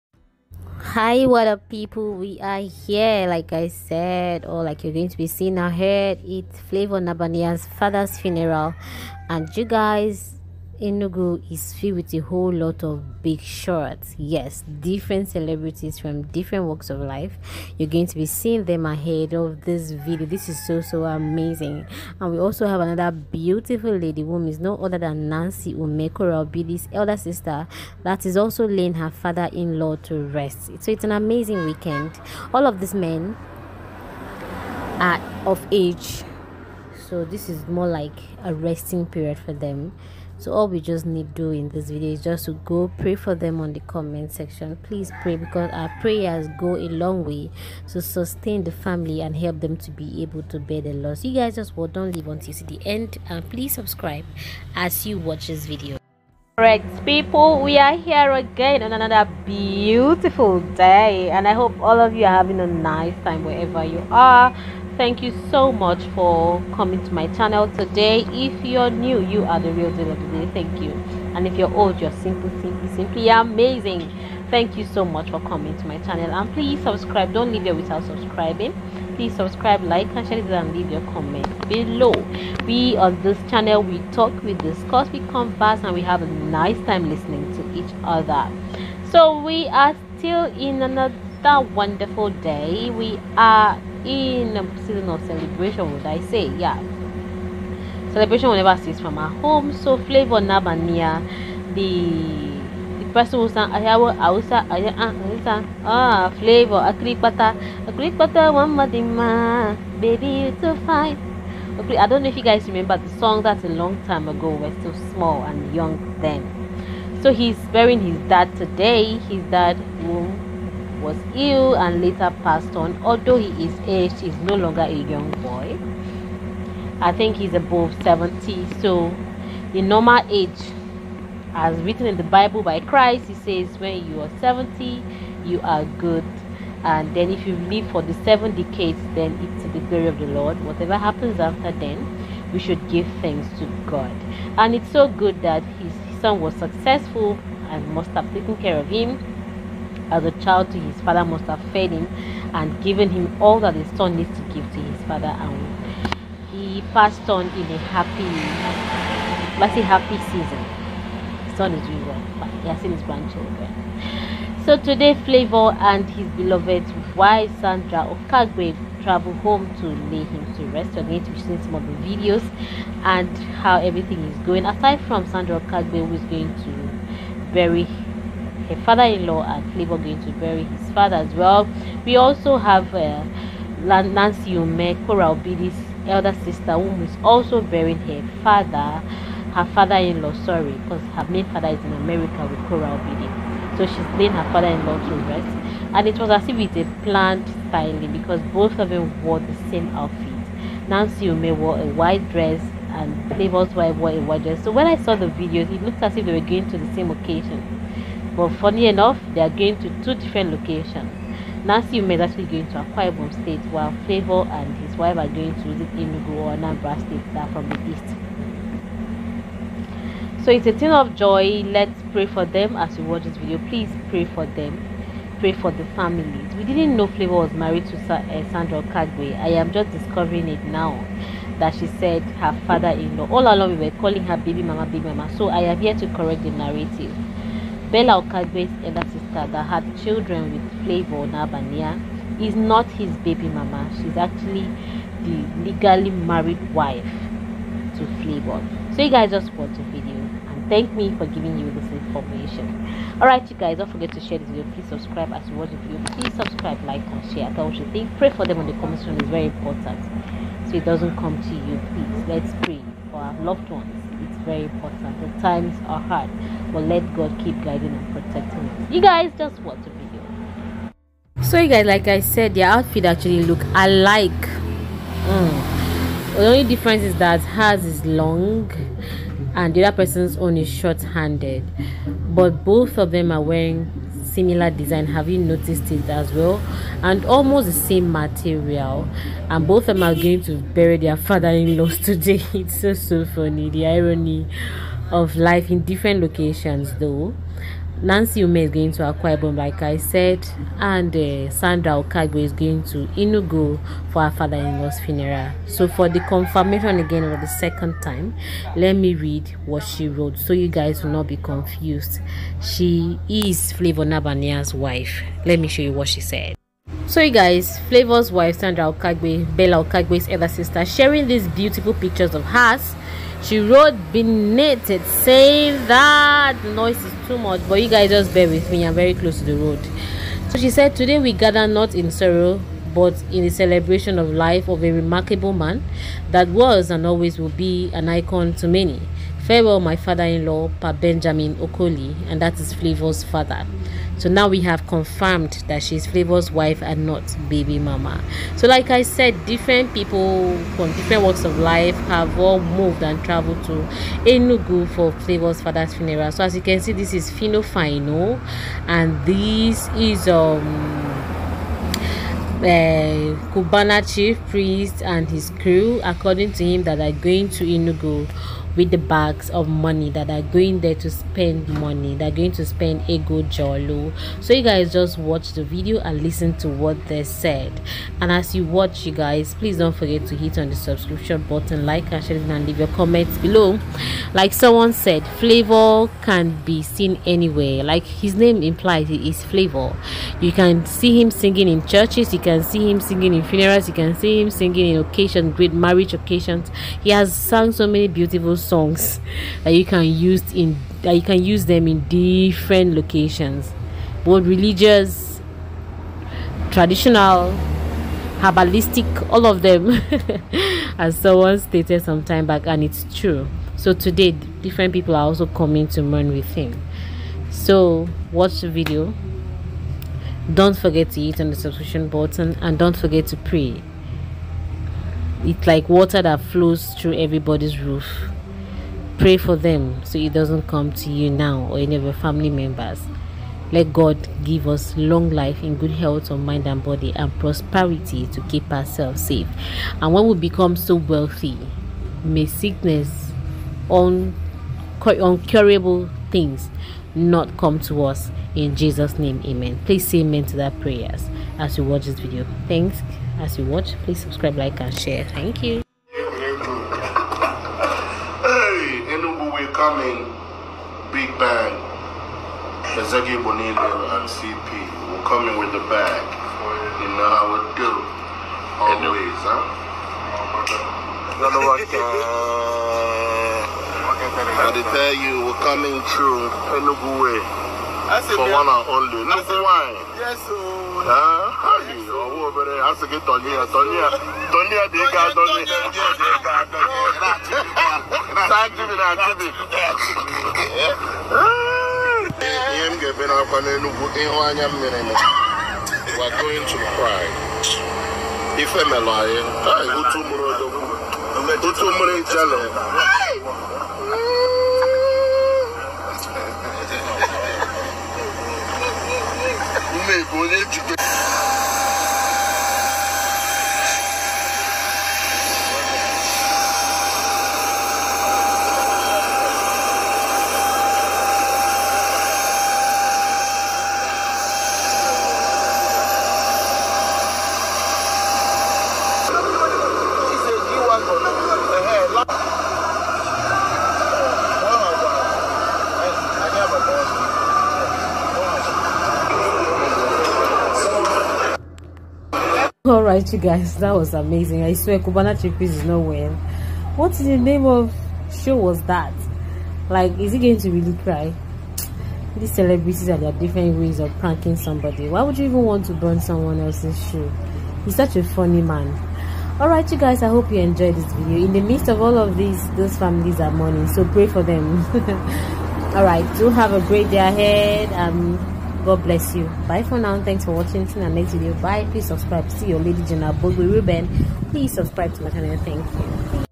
Korra Obidi sister & Flavour nabania wife bella sis travel home to Bury their father inlaw and Dad
Korra Obidi sister and Flavour nabania travel home to Bury their father inlaw and father